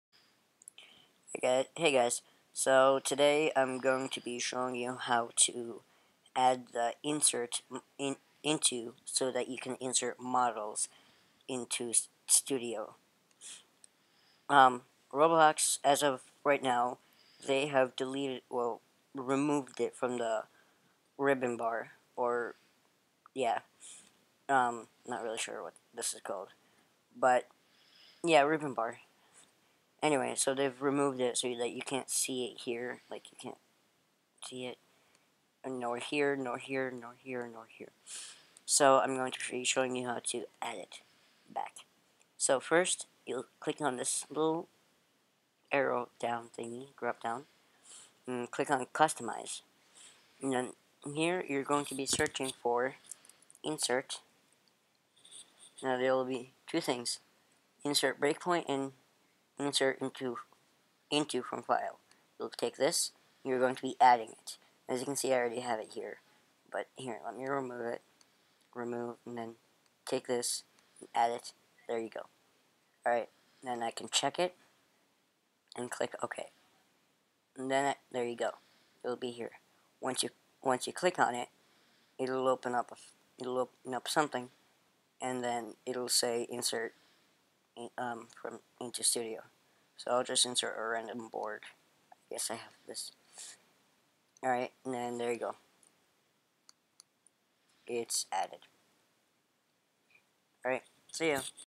How to add models in roblox studio
This will show you how to add models in roblox studio.